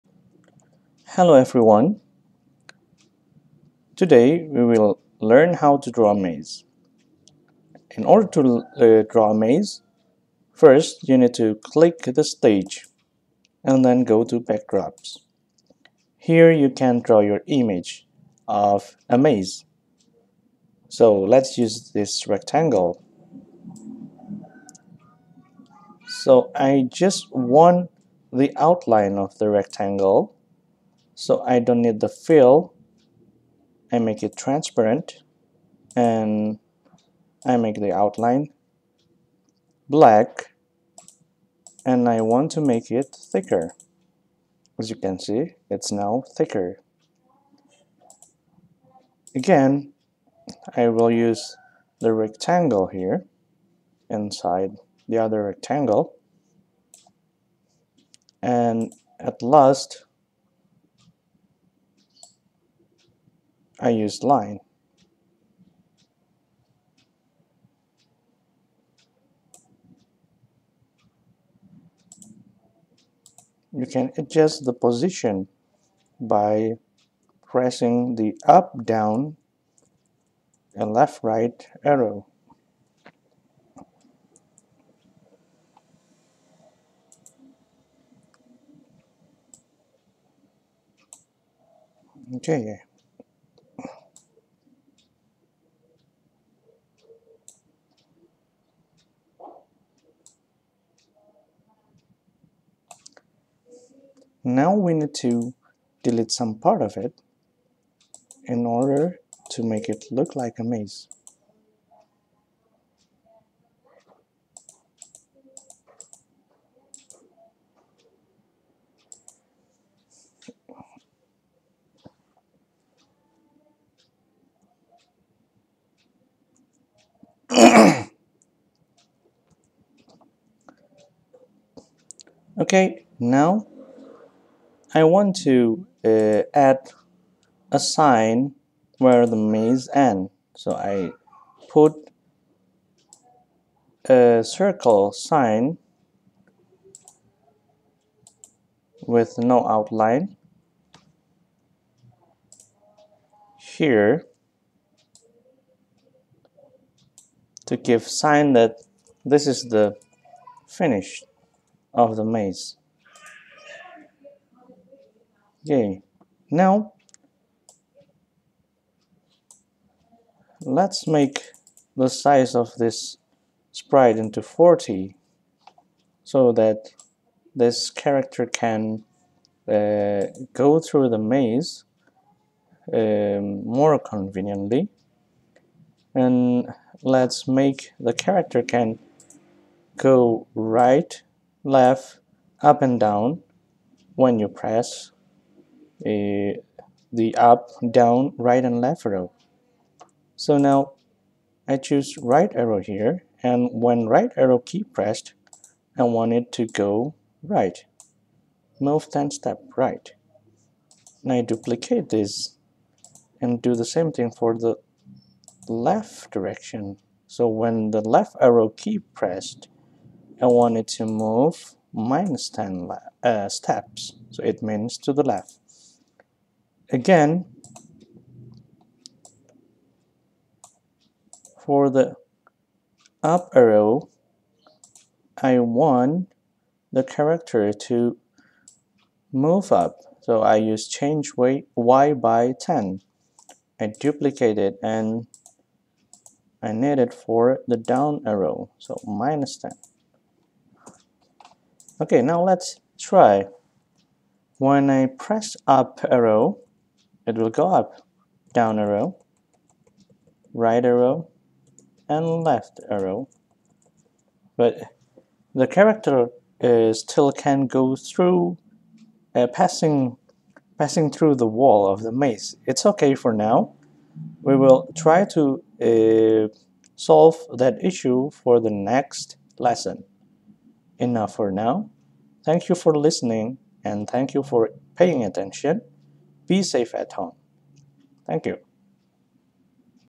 How to draw a maze in scratch
This video demonstrate how to draw simple maze in Scratch